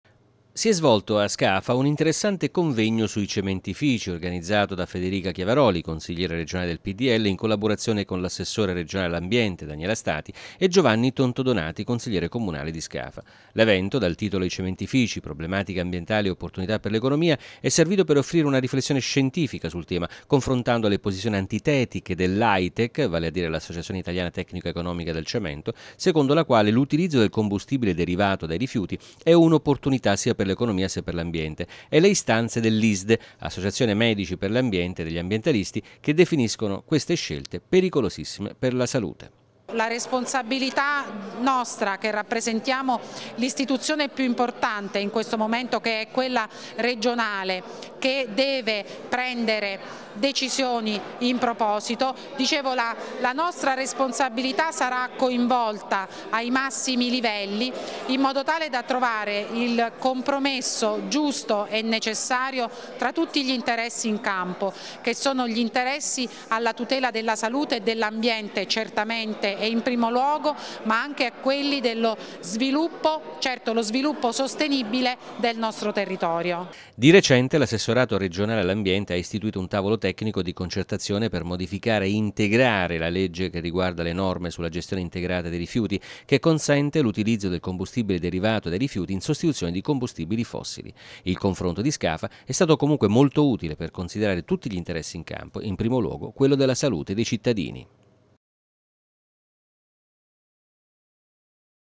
I cementifici e le problematiche ambientali
I cementifici ed il loro impatto sullambiente: di questo si è parlato in un convegno organizzato a Scafa.